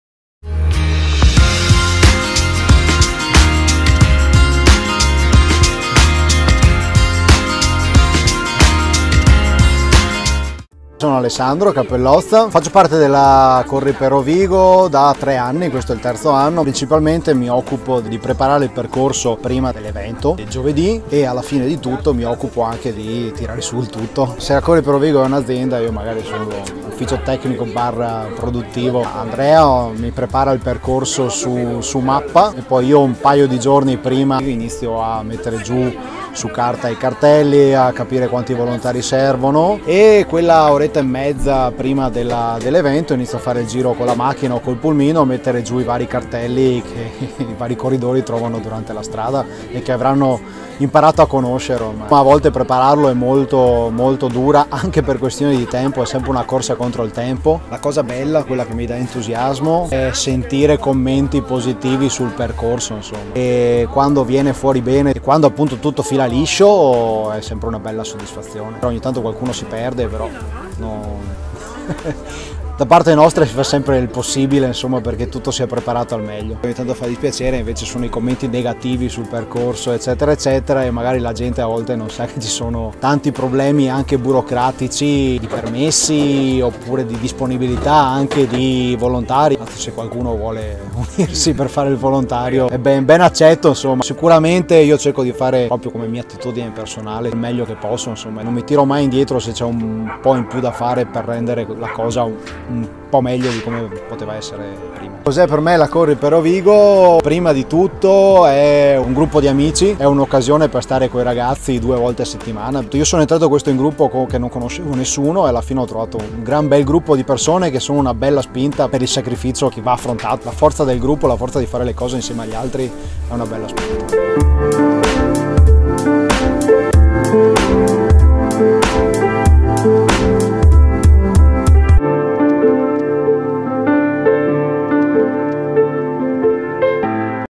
Corri X Rovigo 2017 - BTS #3 "L'allestitore"
Nella quinta edizione di CorriXRovigo, vi vogliamo portare dietro le quinte a conoscere chi c'è e chi ci lavora per rendere questa manifestazione l'iniziativa più amata del polesine.

In questo video: Alessandro Cappellozza

Immagini: Terza tappa, Boara Polesine - 25/05/17